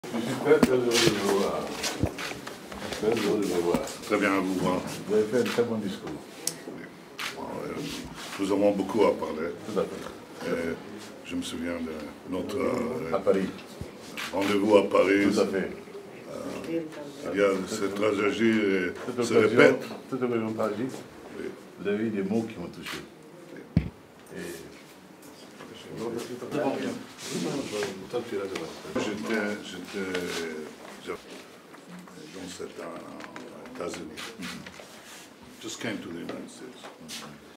PM Netanyahu Meets President of Mali
Prime Minister Benjamin Netanyahu meets with President of Mali Ibrahim Boubacar Keita

ראש הממשלה בנימין נתניהו נפגש עם נשיא מאלי איברהים בובקאר קייטה